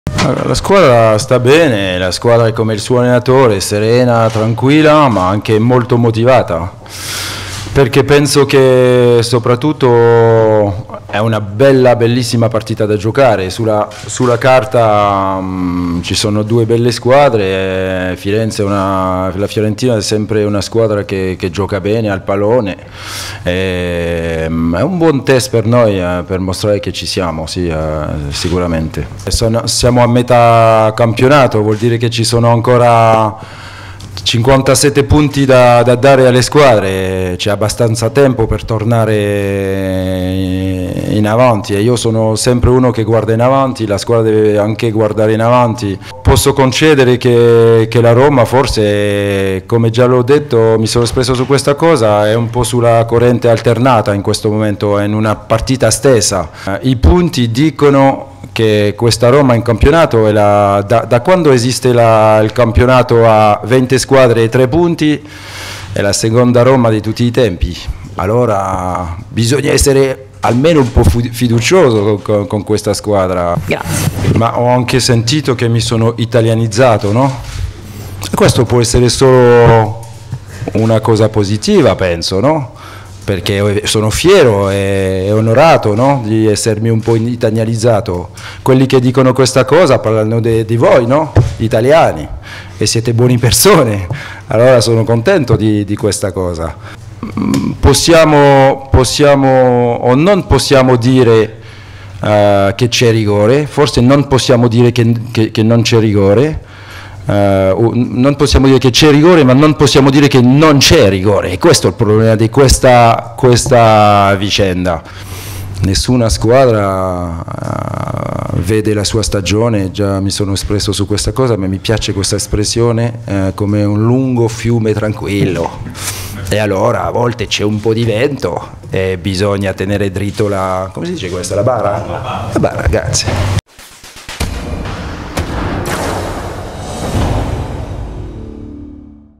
Garcia: "La Roma è motivata, io guardo sempre avanti" [HD]
- Rudi Garcia tiene la consueta conferenza stampa alla vigilia di Fiorentina-Roma: "I punti dicono che la Roma questa è la seconda Roma di tutti i tempi, bisogna essere fiduciosi con questa squadra"